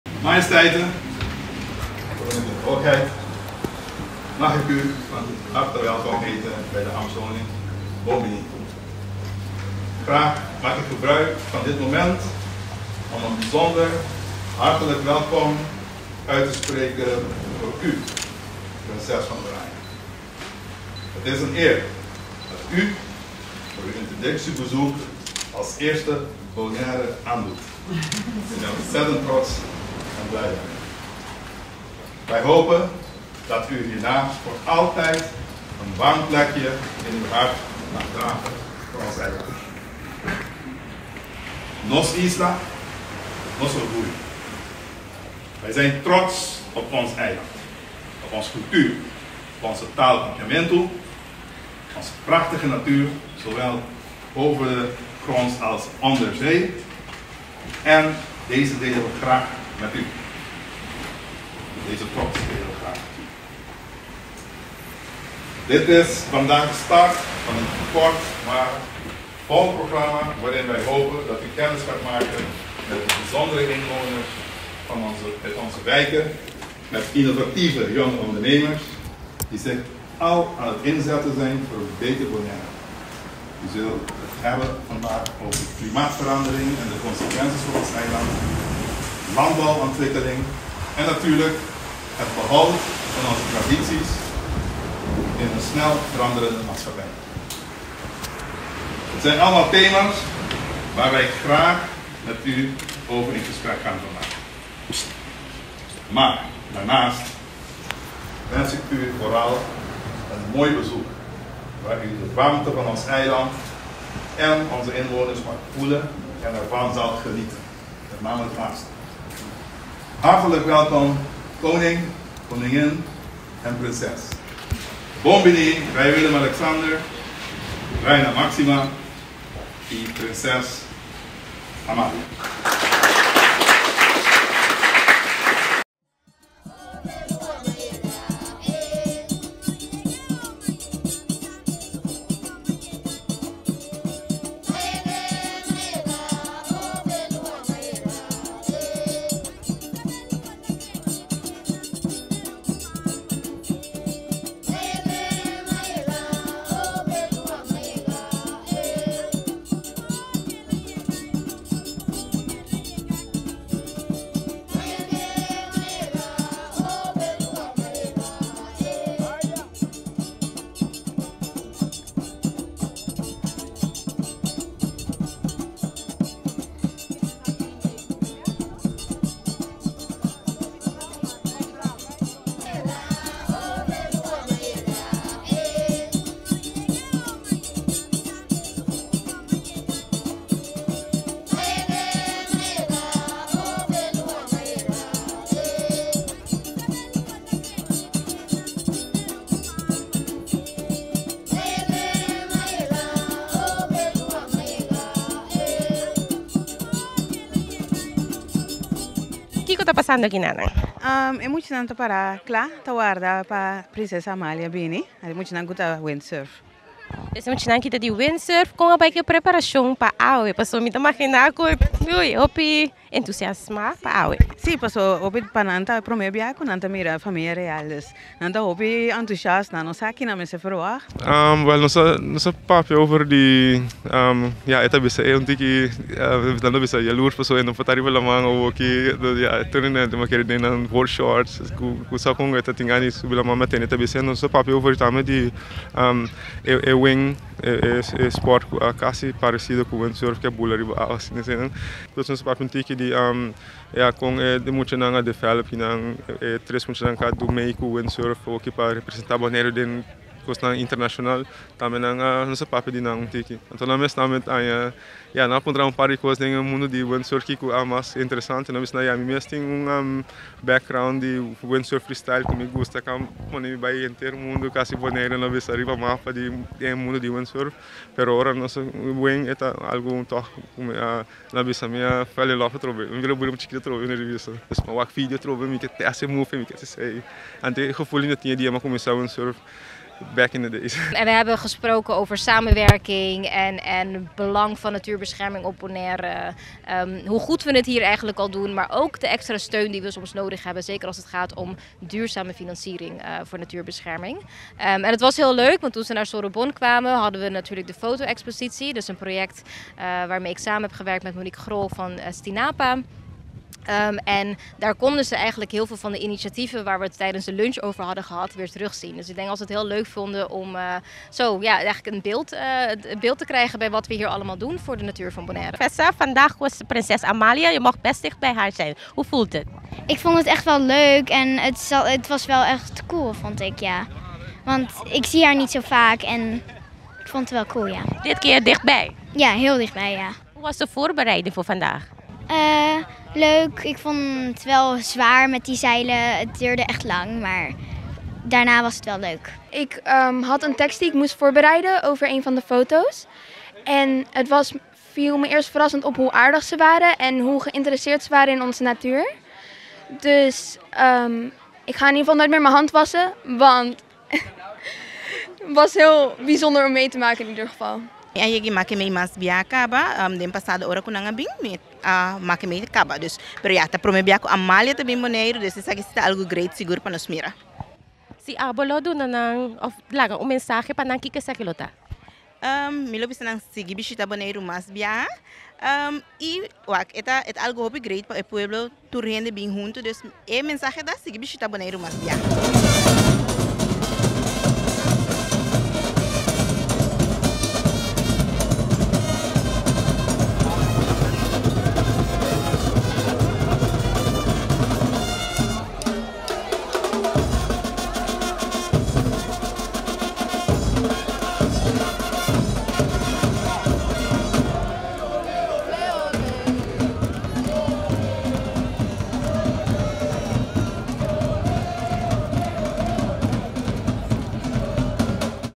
Bishita di famia real na Boneiru
Fin di siman boneiru a risibí bishita di famia real, su mahestat rei Willem-Alexander, su mahestat reina Máxima i su altesa real prinsès van oranje Amalia den kuadro di su bishita na e parti Karibense di reino. Meta di e biahe tabata pa prinsès di oranje sera konosí ku tur seis isla. Notisia Nos TV a kompilá e siguiente reportahe.